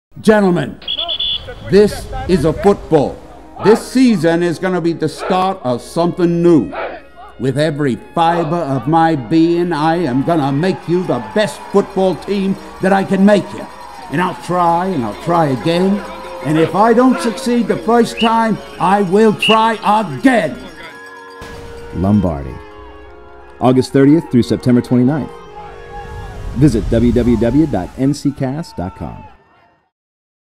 LOMBARDI-"This season is gonna be the start of something NEW."
CAST presents the Regional Premiere of LOMBARDI, by Eric Simonson based on the book, "When Pride Still Mattered- A Life of Vince Lombardi," by David Maraniss.